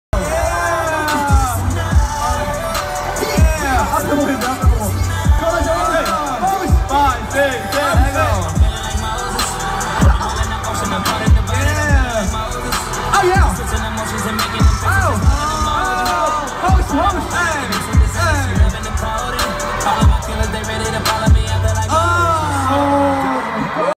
[Fancam] seokmin woozi and seungkwan doing the dougie at auckland fanmeet
not mine